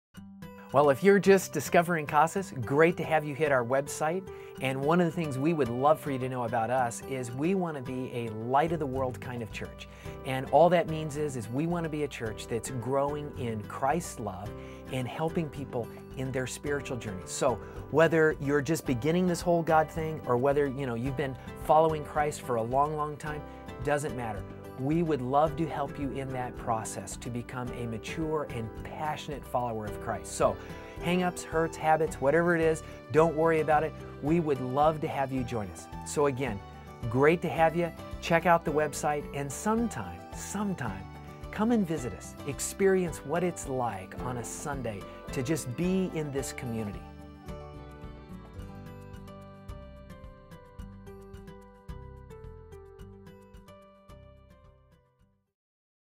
Welcome to The Casas Church Website
An invitation to explore the new Casas Church website, and join us for worship services Sundays.